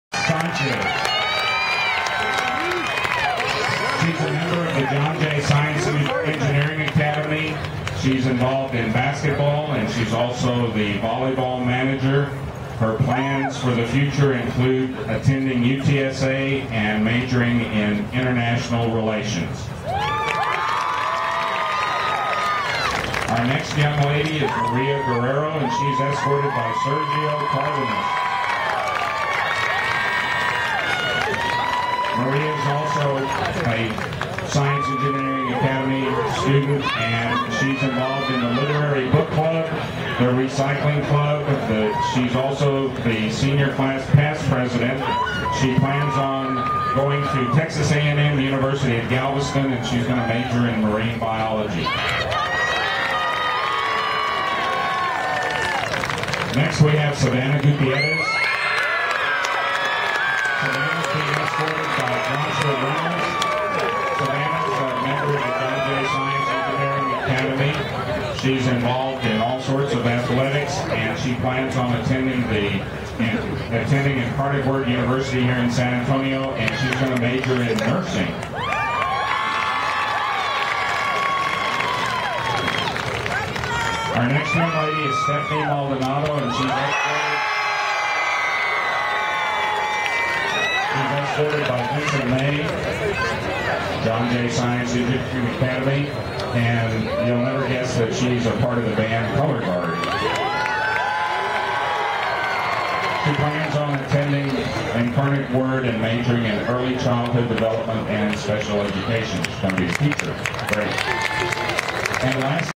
The John Jay Mustang Band performs at the pep rally for the Stevens football game on Friday, October 5, 2012 (Homecoming).